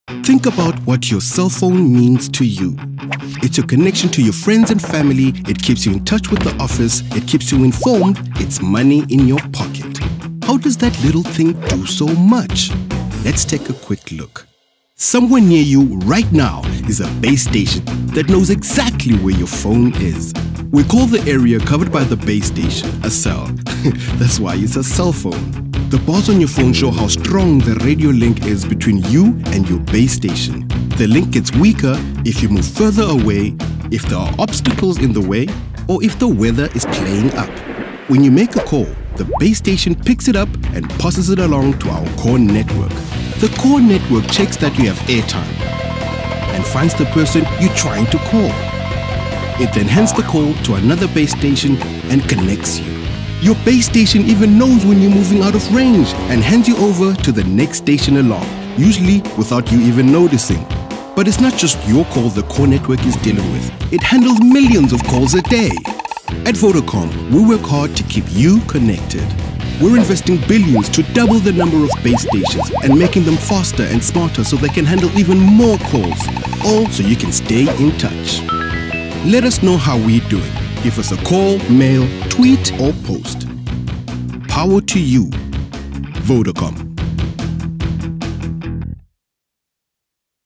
Vodacom Self Service | Behind the scenes on the Vodacom Network
Ever wondered what happens on our network when you make a call - here's a brief illustration of that complicated process!